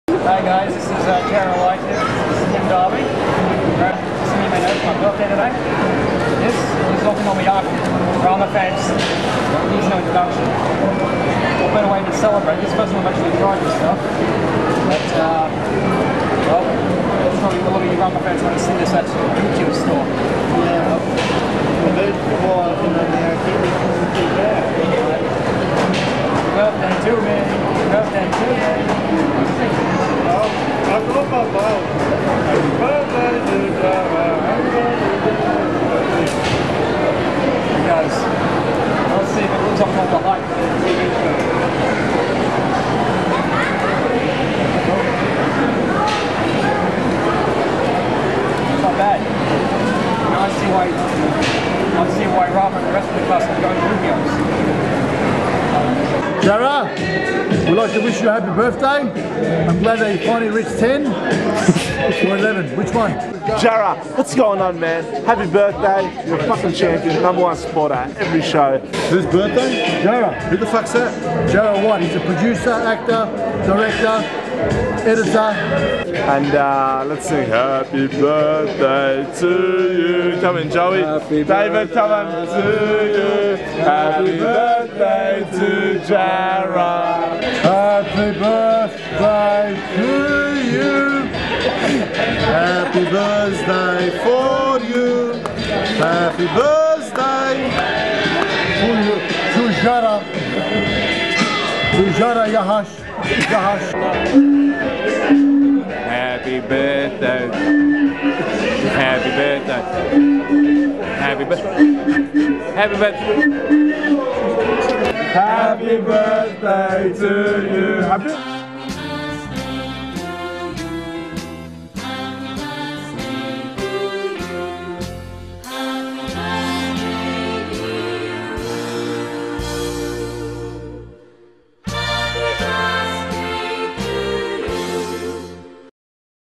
Jarrah's birthday
As some of you know, last night (April 14th) was my birthday. I want to say thanks to all my many friends and subscribers who sent me their kind regards on Youtube, Facebook, DeviantArt and elsewhere on the internet.

In return I thought I'd share some video highlights from my very special day. It certainly was quite eventful. Tim Darby and I met up in town at this new sushi place which also sells hot food, including okonomiyaki (a Japanese cross between pancakes and pizza). Fellow fans of Ranma 1/2 might remember it from the series, though I'm pretty sure they never put birthday candles on it. :P

There was a lot of ambient noise in the food court. Despite my best efforts to filter it out, it's still a little hard to hear. So I put in subtitles. Hope no one minds. ^^;

Speaking of pizza, later that night I met up with some of the lads from the comedy series Fat Pizza. Tahir Bilgic (Habib), Rob Shehadie (Rocky), Anthony Salame (Omar), and Hung Lee (the Vietnamese crime boss).

 All in all, a birthday to remember. Special thanks goes out to all who made it so memorable. :)